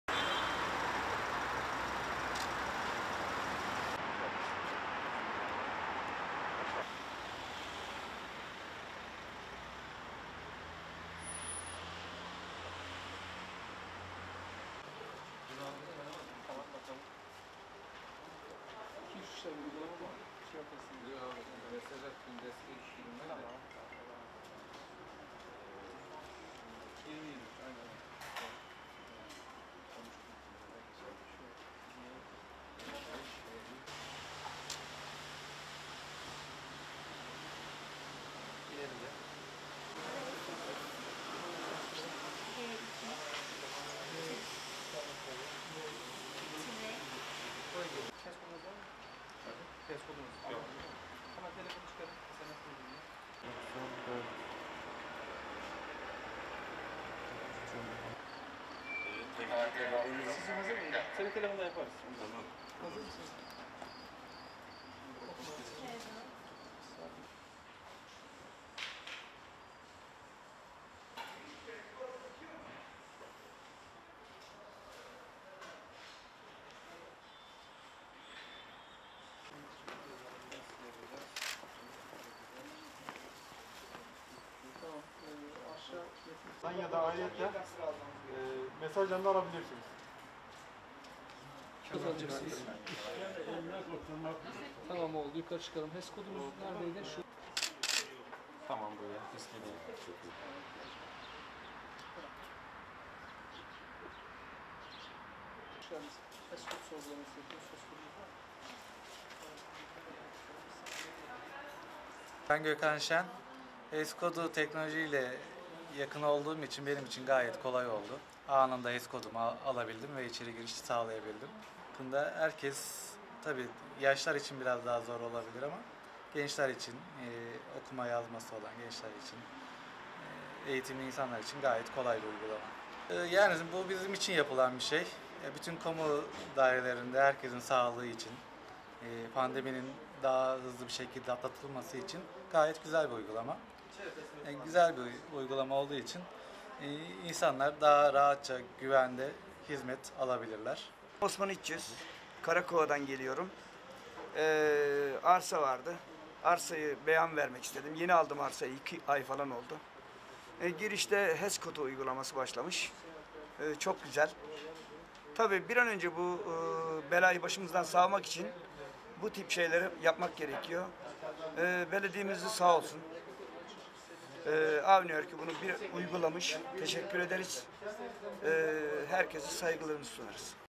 PAMUKKALE BELEDİYESİ’NE HES KODU OLMAYAN GİREMİYOR
İçişleri Bakanlığı, koronavirüs pandemisini önlemek amacıyla kamu kurum ve kuruluşlarında HES kodu uygulamasını zorunlu hale getirdi. Pamukkale Belediyesi, hafta başından bu yana HES kodu kontrolünü sağlıyor.